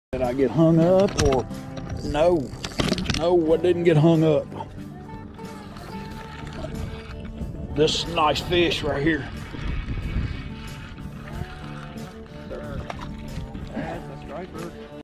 THESE Fish FIGHT BIGGER than they are!
This was a trip out on the Cumberland River upstream of Old Hickory Lake to do a little Winter fishing. Turned out to be a pretty decent multi-species day. This was my 3rd fish of the day and these Striped Bass are always fun to catch. They definitely fight bigger than they are.